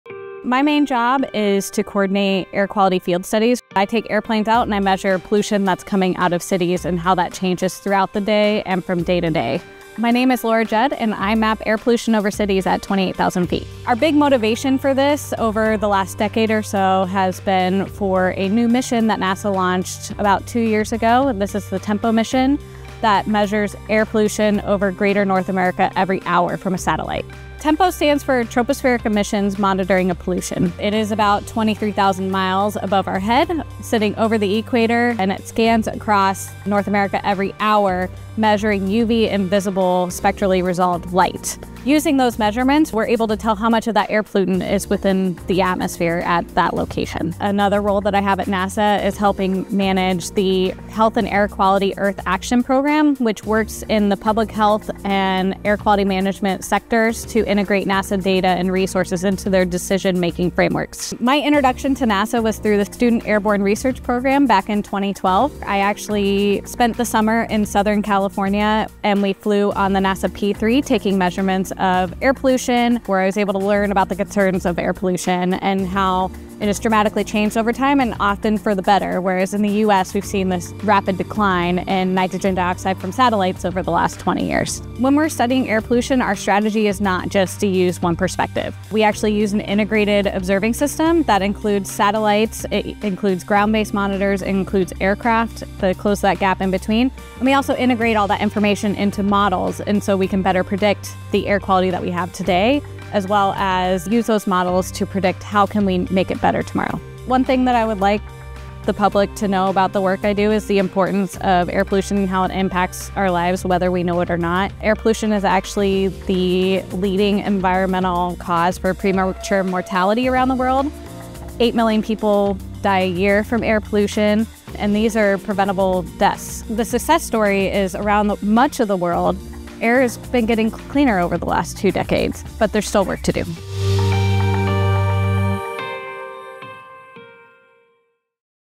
Faces of Technology: Meet Laura Judd
Meet Laura Judd, a research project scientist at NASA's Langley Research Center. Laura maps air pollution over cities at 28,000 ft.as part of an integrated observing system to better understand air quality. Working with NASA's Health and Air quality Earth Action program, she and others on the team help public health and air quality managers use NASA data to make informed decisions about the air we breathe. To learn more about how NASA uses air quality data to solve today's challenges,